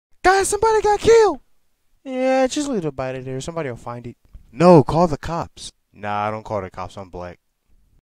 Don't call the cops
We all know whats gonna happen if you do

Characters from SR, a game and animated series my friend is working on.